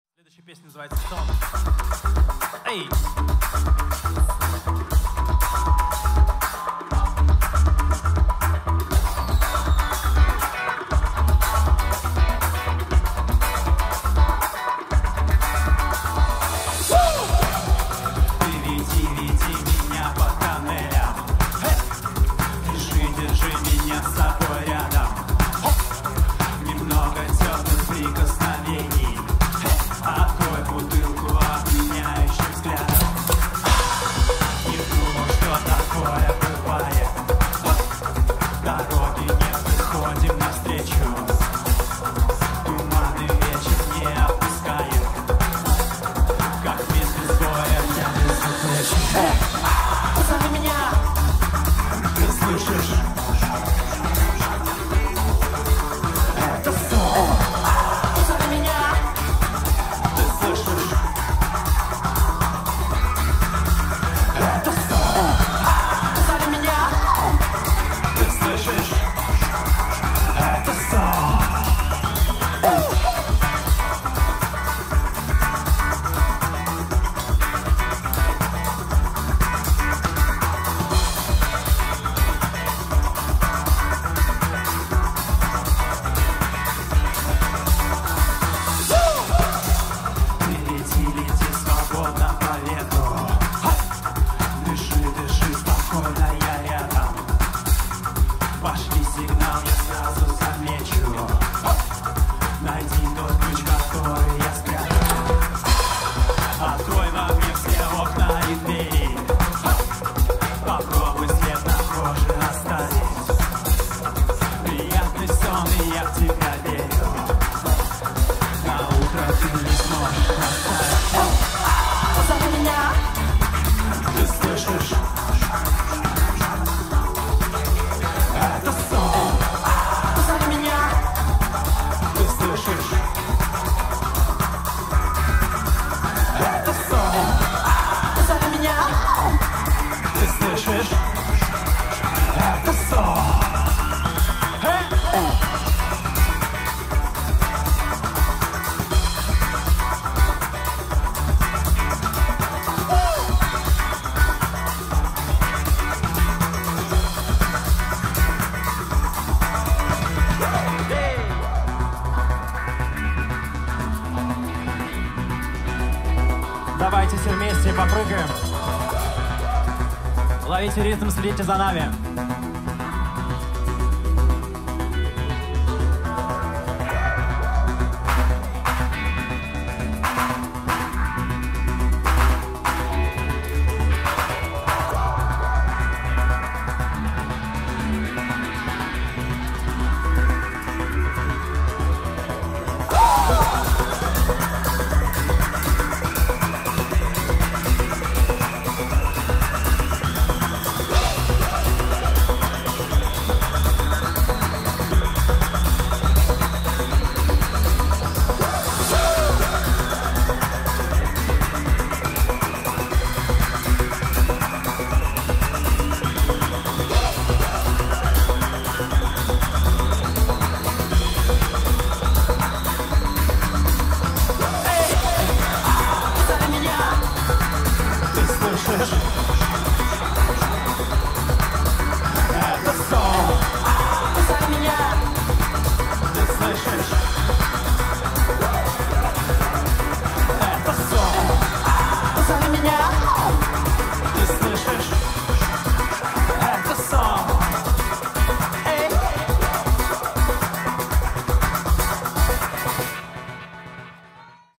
Сон. Electric Lines
Сон. Electric Lines / Электрические Линии. Live Set - SPb. Санкт-Петербург, Zoccolo 2.0, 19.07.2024. Живой звук.

Яркая и энергичная электронная музыкальная группа со страстью к созданию мощных ритмов и незабываемым выступлениям.